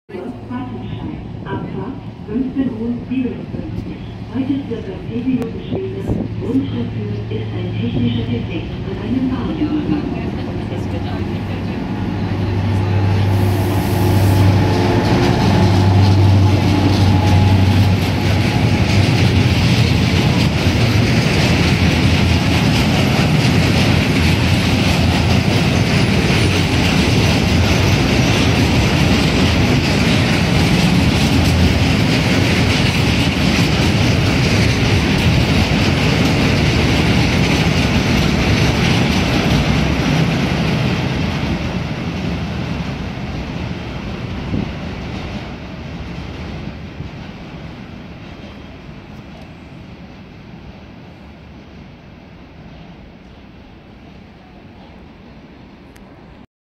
A bit of heavy diesel haulage, provided by the Baureihe 294 in freight service - Passage -
A first on my channel: A bit of freight action!

Provided by the MaK built BR 294 of TOC DB Cargo we see some heavy diesel duty coming through the central station of Bochum!

Honestly freight trains aren't my specialty, so other then carrying some raw materials, I couldn't tell you what the shipment of this train included. Perhaps you guys can fill me in?

Anyhow I hope you enjoy! 😄

Cheers! 🍀